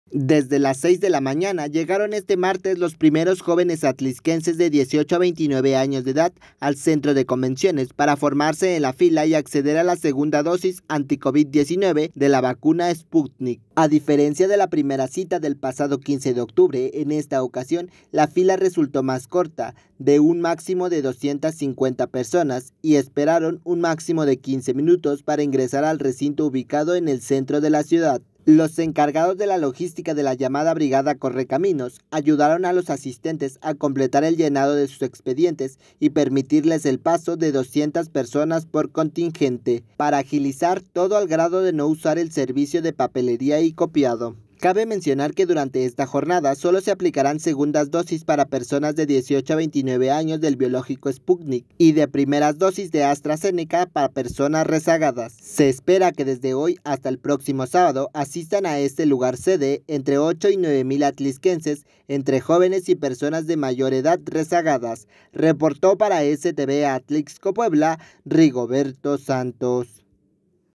🟣RESPONDEN JÓVENES DE ATLIXCO A SEGUNDA DOSIS ANTI COVID
Desde las seis de la mañana llegaron este martes los primeros jóvenes atlixquenses de 18 a 29 años de edad al centro de convenciones para formarse en la fila y acceder a la segunda dosis anti Covid de la vacuna Sputnik.